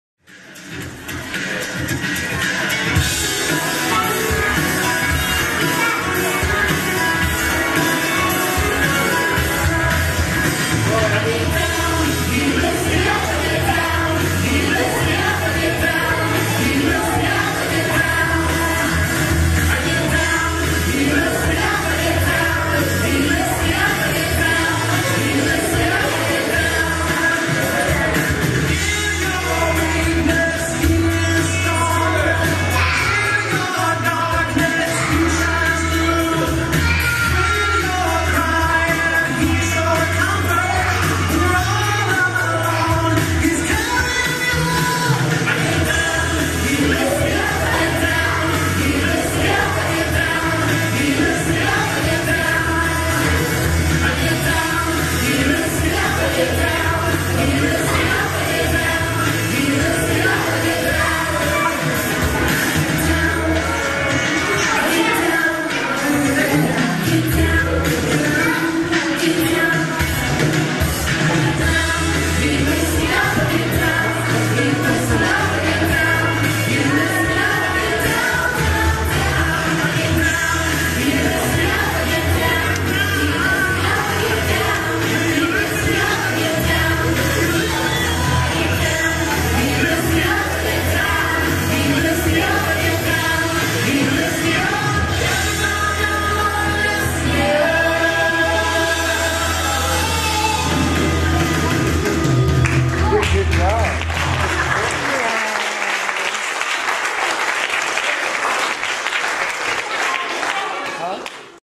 2009 07 12 VBS GSO 01 MOV
Daniel, Abby and Ella Chewning participate in a Vacation Bible School program in Greensboro NC